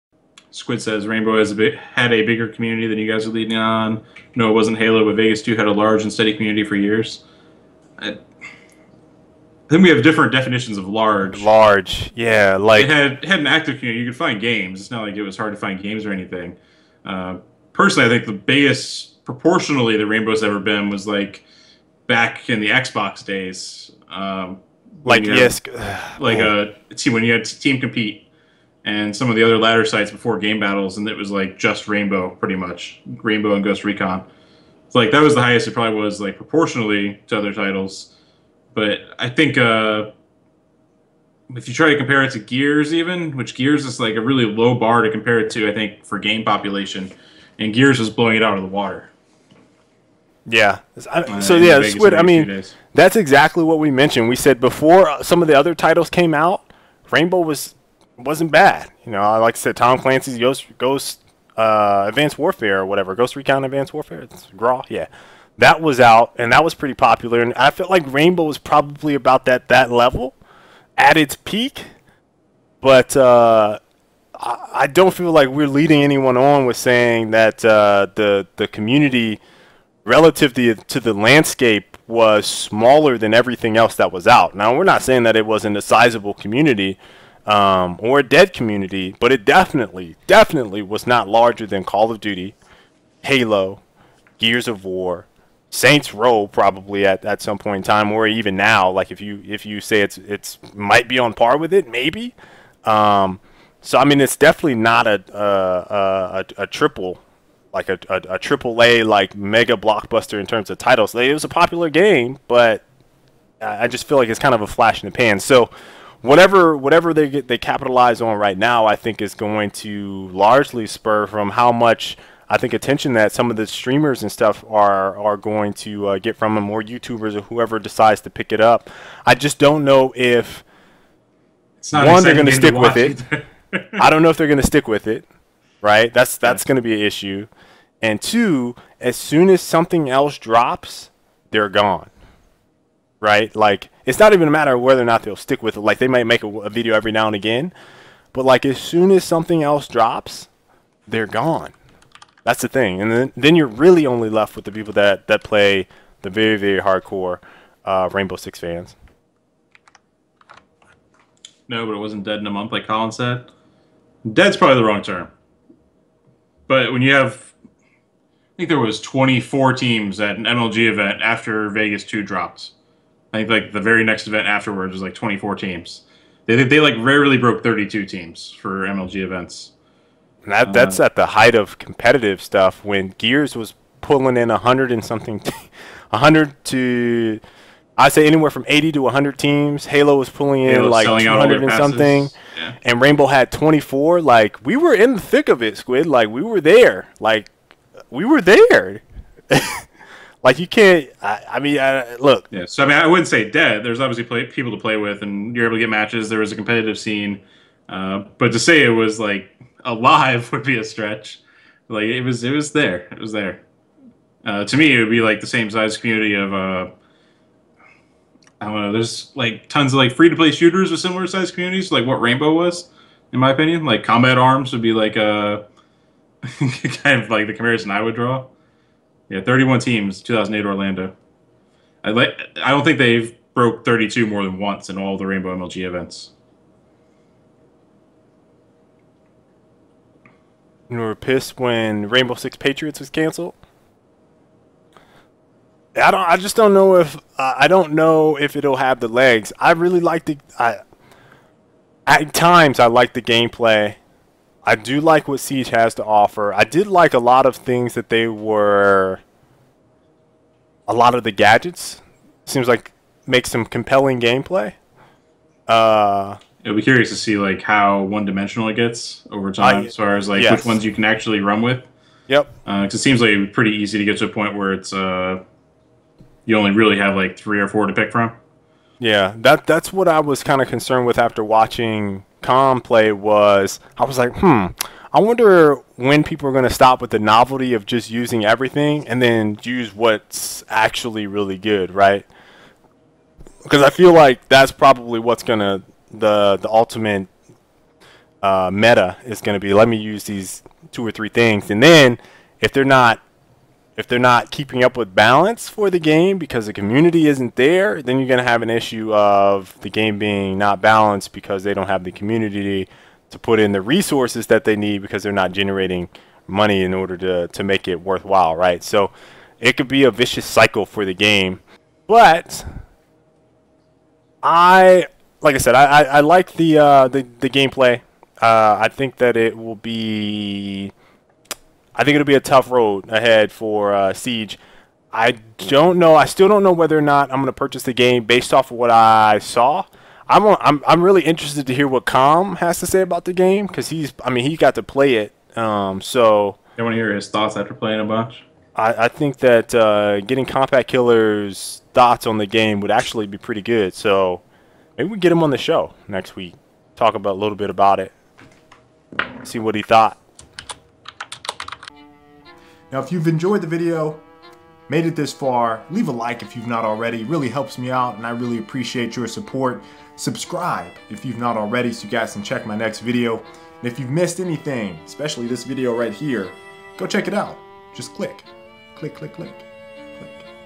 Initial Impressions of Rainbow Six Siege Alpha Gameplay (Frag Logic #114 Gaming Podcast)
We discuss our initial impressions of Rainbow Six Siege from live, streamed gameplay on Twitch TV.